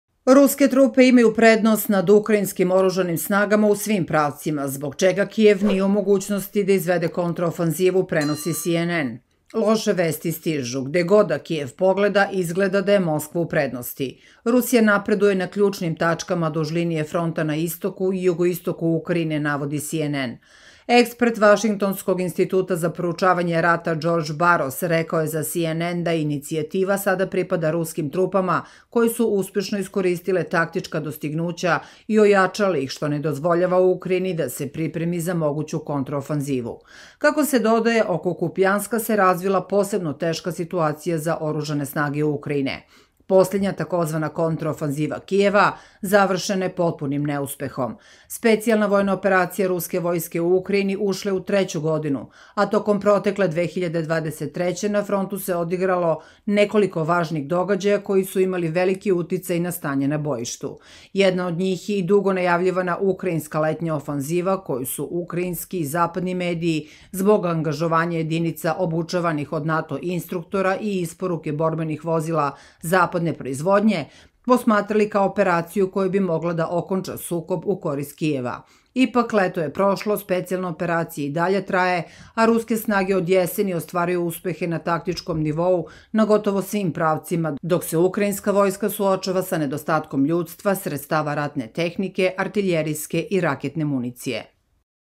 Si-En-En priznao: Ruska vojska je u prednosti u svim pravcima na frontu
Ruske trupe imaju prednost nad ukrajinskim oružanim snagama u svim pravcima, zbog čega Kijev nije u mogućnosti da izvede kontraofanzivu, prenosi Si-En-En.